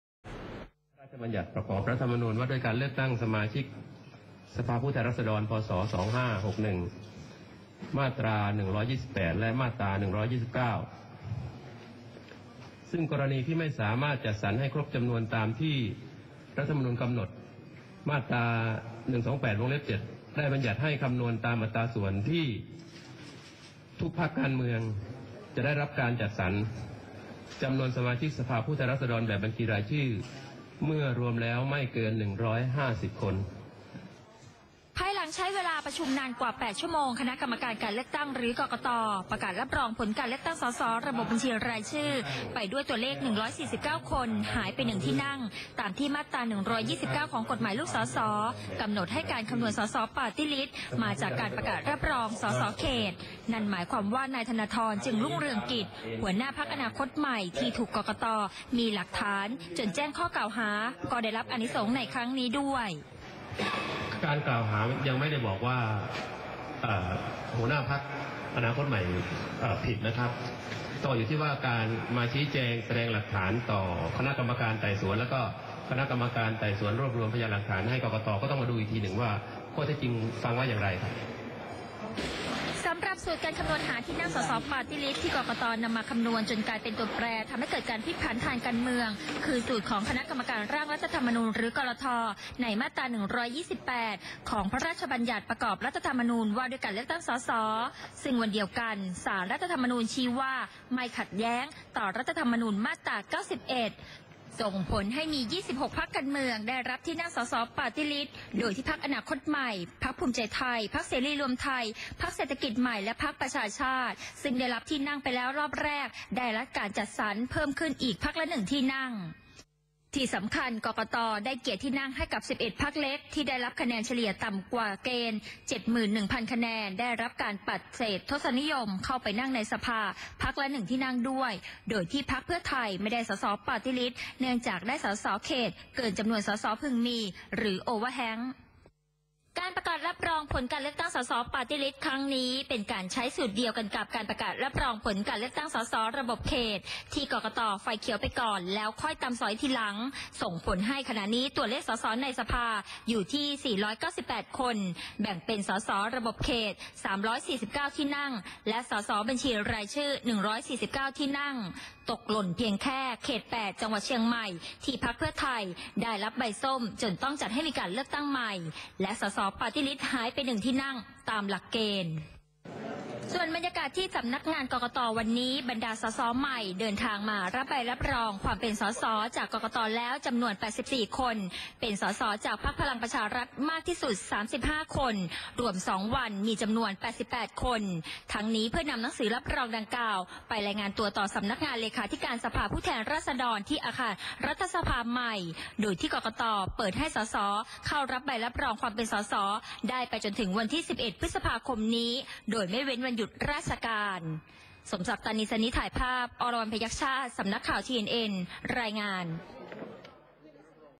กกต.รับรอง ส.ส. 498 คน เดินหน้าการเมือง | 8 พ.ค.62 | TNN ข่าวดึก
คณะกรรมการการเลือกตั้งได้ประกาศผลการเลือกตั้ง สส. อย่างเป็นทางการ ทั้งแบบแบ่งเขต 349 คน แบบบัญชีรายชื่อ 149 คน รวมแล้ว 498 คน เกิน 95 เปอร์เซนต์ ทำให้หลังจากนี้เดินหน้าเข้าสู่การจัดตั้งรัฐบาล